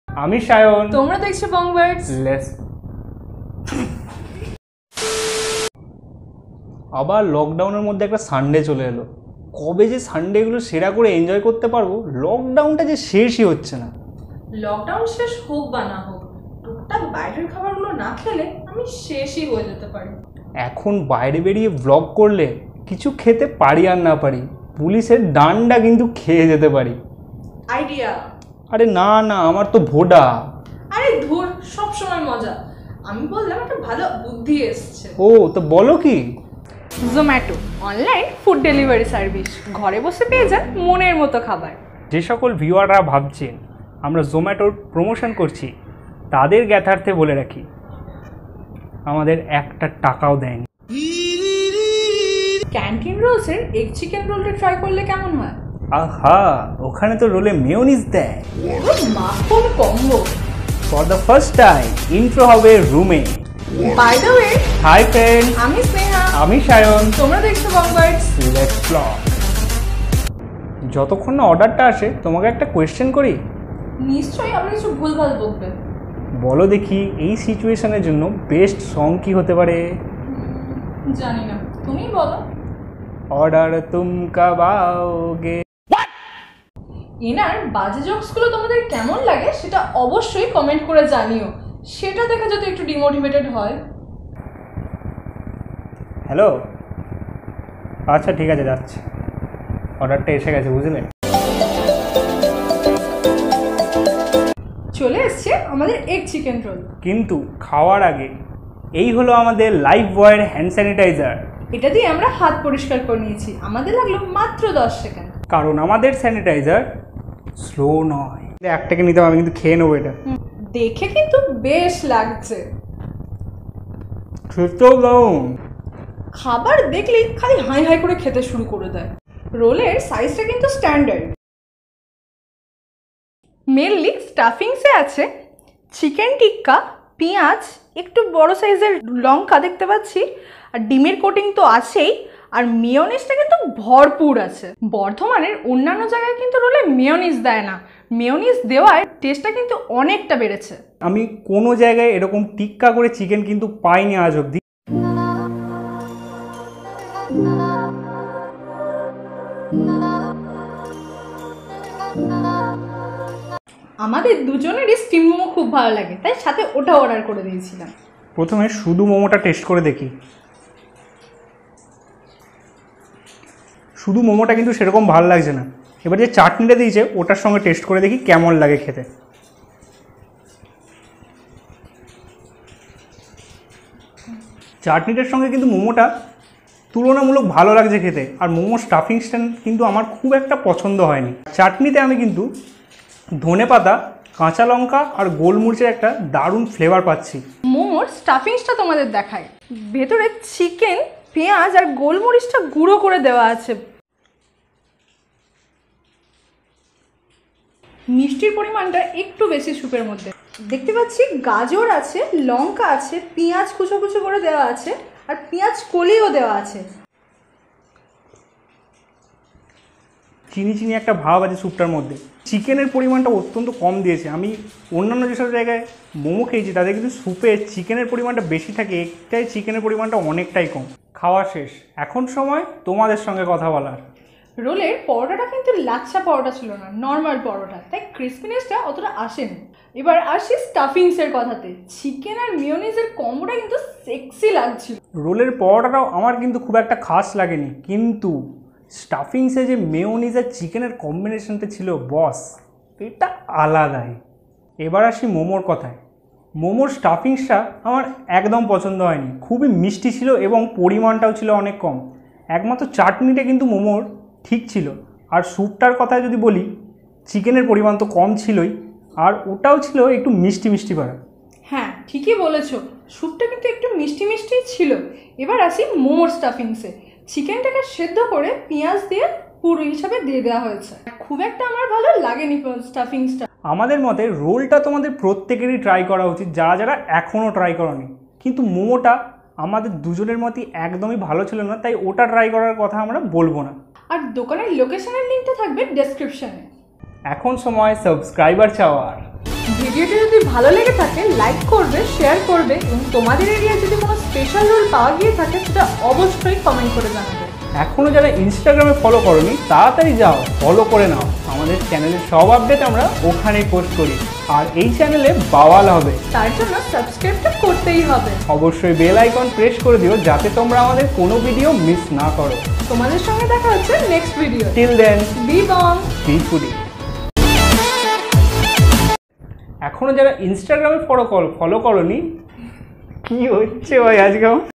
Purba Bardhaman এর Roll specialist ||Bardhaman Canteen's Roll||বাংলা vlog
আজকের ভিডিও রোল specialist Canteen's Roll এর Egg Chicken Roll নিয়ে.
সঙ্গে থাকছে আর একটি স্পেশাল item😋

Topic discussed:
1.roll specialist
2.egg chicken roll
3.chicken momo
4.canteen roll's
5.bardhaman food vlog
6.street food bardhaman
7.বাংলা vlog বর্ধমান